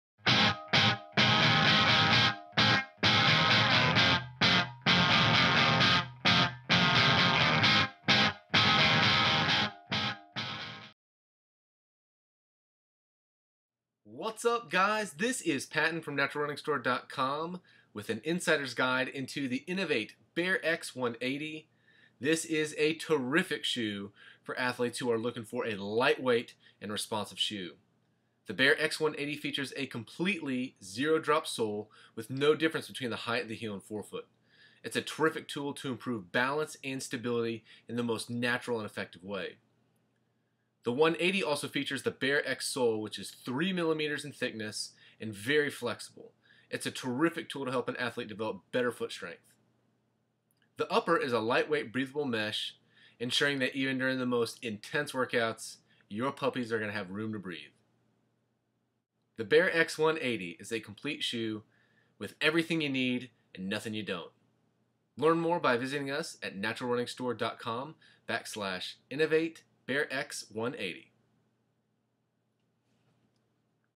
Inov8 Bare X 180: Insider's Guide From NaturalRunningStore.com
The Inov8 Bare-X 180 is a zero drop running and training shoe with a flexible sole and lightweight breathable upper.  Learn more about this natural running and crossfit shoe